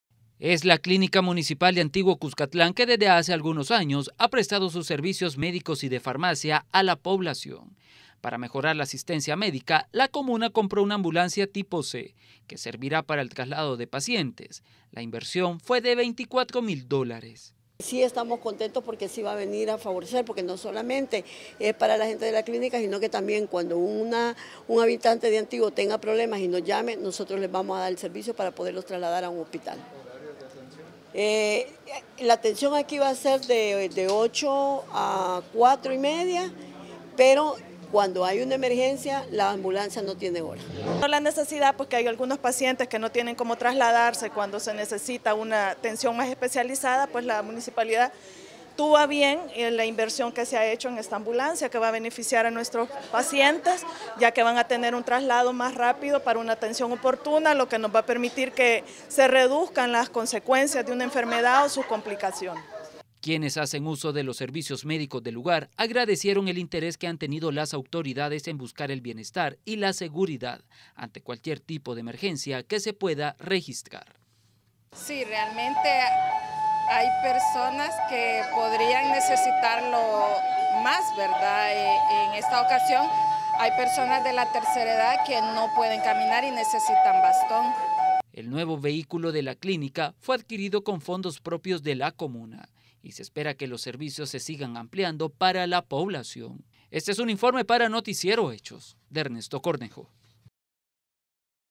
Clínica municipal
La clínica comunal de Antiguo Cuscatlan compro una nueva ambulancia para el traslado de pacientes